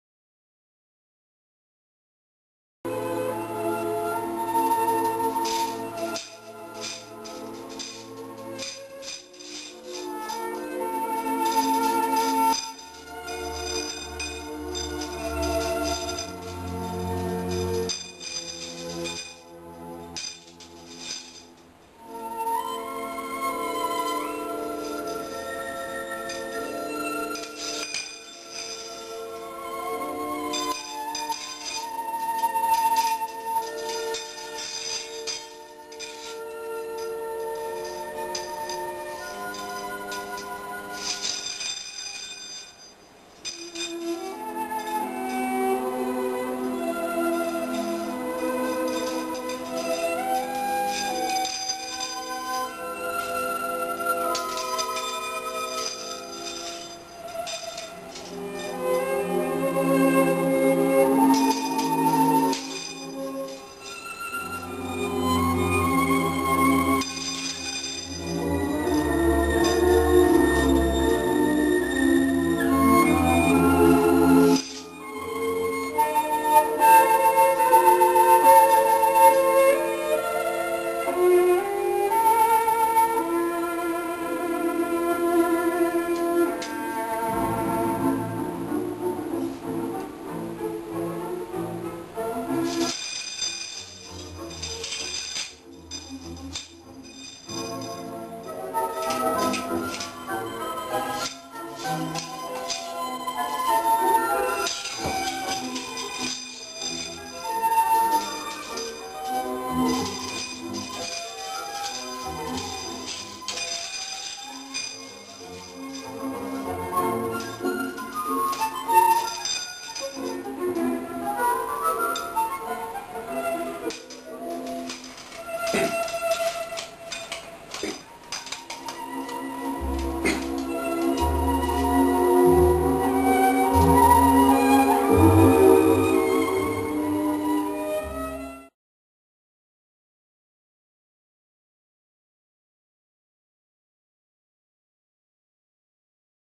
The Eternal Linking Rings
A timeless classic of magic, this version done with eight rings and a solo performer